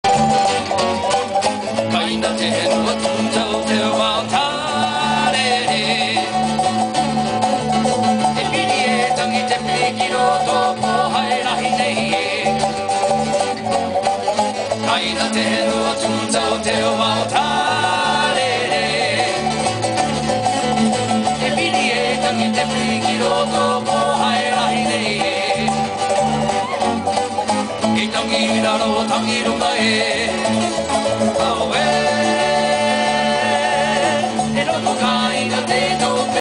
Sunset at kauai beach
Dancers at sunset on Kauai beach 2007